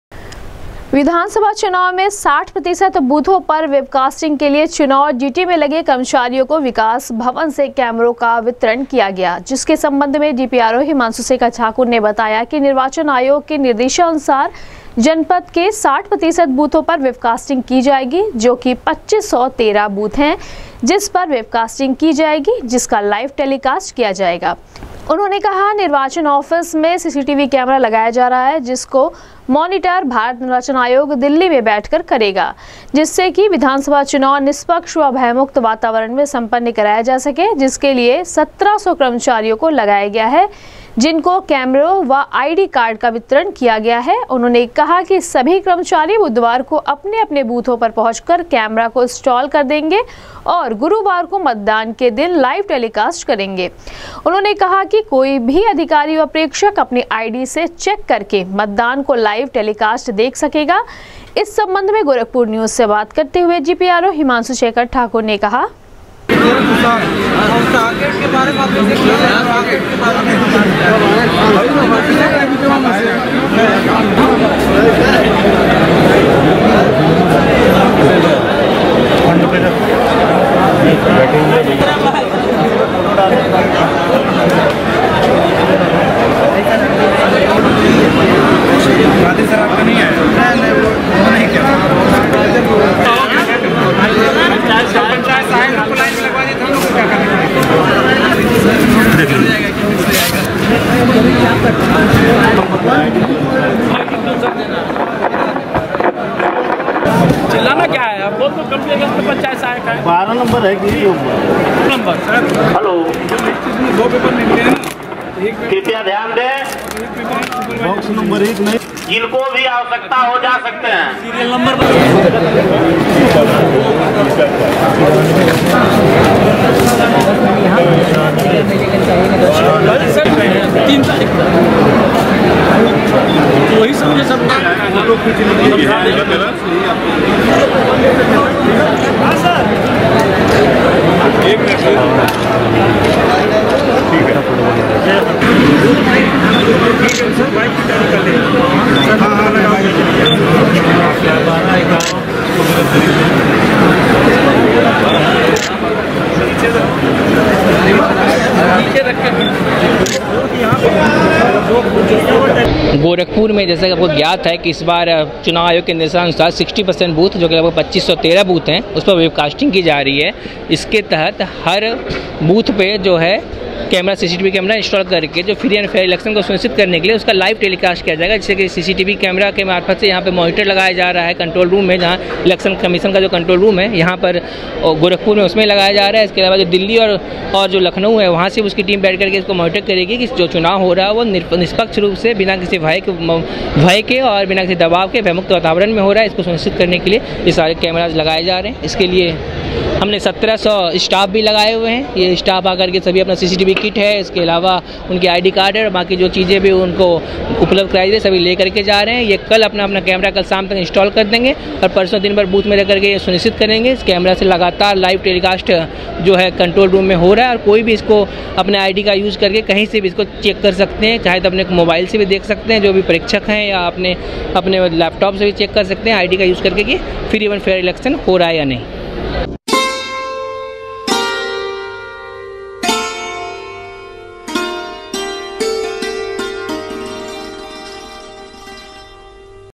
60 प्रतिशत बूथों पर वेबकास्टिंग के लिए चुनाव ड्यूटी में लगे कर्मचारियों को कैमरो का किया गया वितरण
60 प्रतिशत बूथों पर वेबकास्टिंग के लिए चुनाव ड्यूटी में लगे कर्मचारियों को कैमरो का किया गया वितरण,डीपीआरओ ने कहा, 25 सौ 13 बूथों पर की जायेगी वेबकास्टिंग,सभी कर्मचारी बुधवार को अपने निर्धारित बूथों पर पहुँचकर कैमरों को करेंगे इंस्टॉल,गुरुवार को मतदान का करेंगे लाइव टेलीकॉस्ट